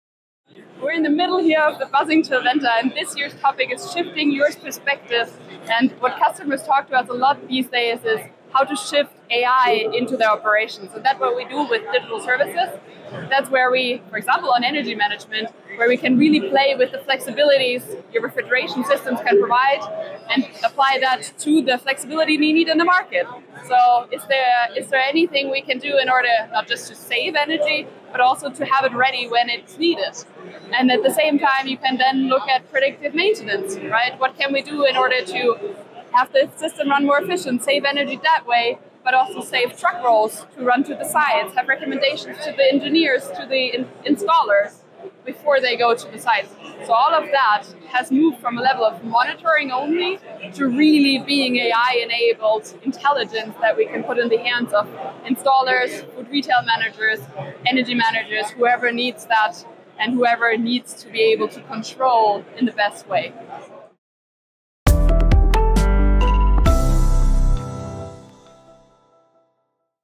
AI for Energy Savings and Flexibility | Chillventa 2024
Join us at Chillventa 2024, where we're exploring how AI is transforming the refrigeration industry! This year's focus is on shifting perspectives and helping our customers integrate AI into their operations. Discover how our digital services can enhance energy management by maximizing the flexibility of refrigeration systems and providing predictive maintenance solutions. Learn how AI-driven insights can help installers, engineers, and energy managers save energy, reduce costs, and optimize performance—taking refrigeration from simple monitoring to intelligent, data-driven decisions.